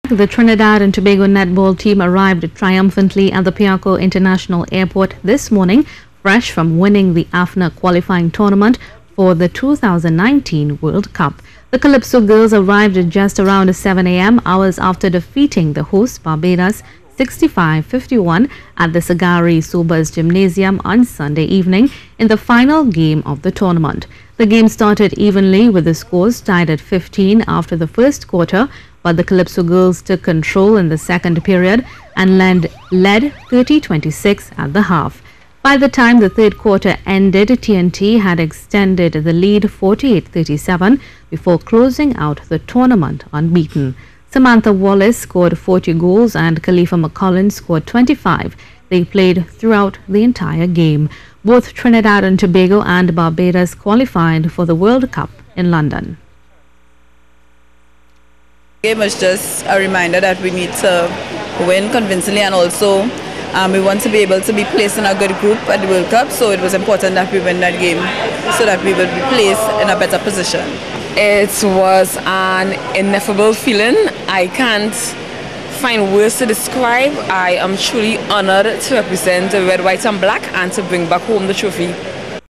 Victorious Netball Team Returns Home
The Trinidad and Tobago Netball Team arrived triumphantly at the Piarco International Airport this morning, fresh from winning the AFNA Qualifying Tournament for the 2019 World Cup.

The Calypso Girls arrived just around 7am, hours after defeating the hosts, Barbados, 65-51 at the Sir Gary Sobers Gymnasium on Sunday evening in the final game of the tournament.